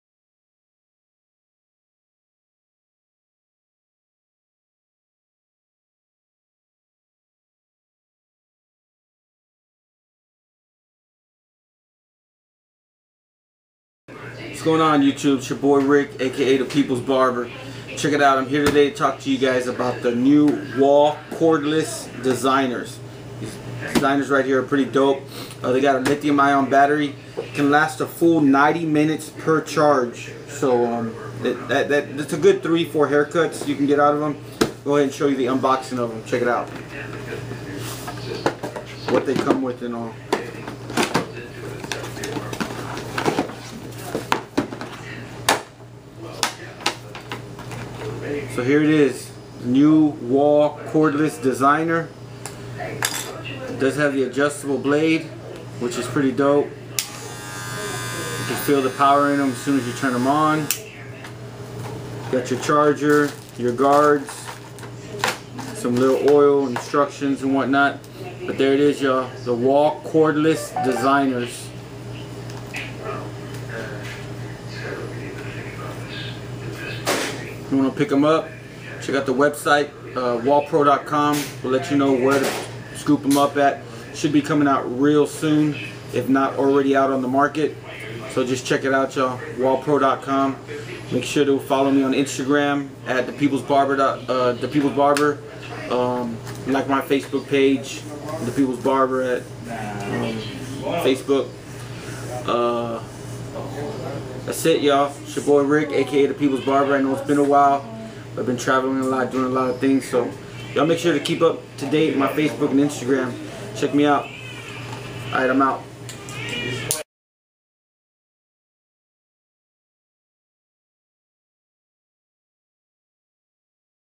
Unboxing of the Wahl Cordless Designers - DAPEOPLESBARBER.COM
This is a quick video of Wahl Educator Rick "Da People's Barber" Morin unboxing the all new Wahl Cordless Designers.